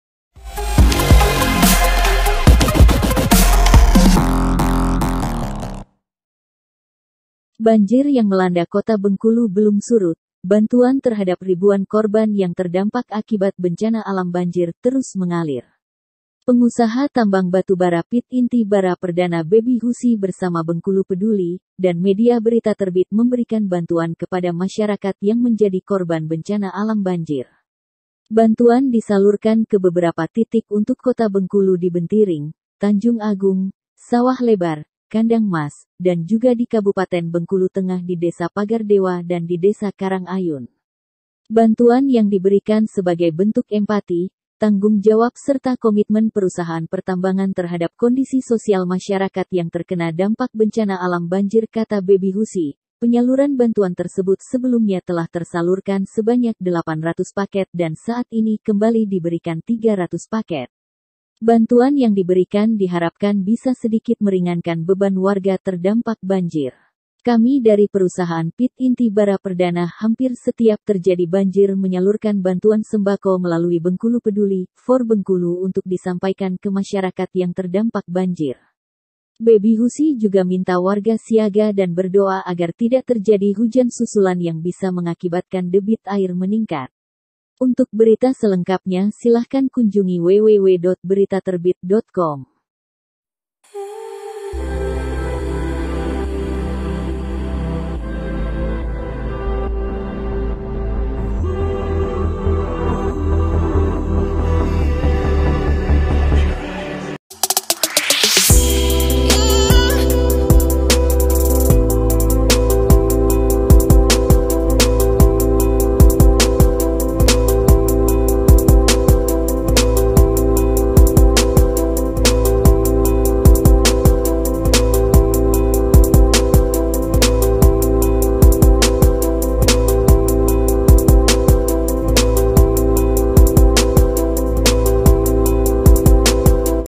PT. Inti Bara Perdana Bersama Bengkulu Peduli dan Media Beritaterbit Salurkan Bantuan Korban Banjir
Bengkulu, beritaterbit.com – Banjir yang melanda Kota Bengkulu belum surut, bantuan terhadap ribuan korban yang terdampak akibat bencana alam banjir terus mengalir, Jumat (2/9/2022).

Pengusaha Tambang Batubara PT. Inti Bara Perdana Baby Husie bersama Bengkulu Peduli, dan media Beritaterbit.com memberikan bantuan kepada masyarakat yang menjadi korban bencana alam banjir.

Editor : Rifky Ardiansyah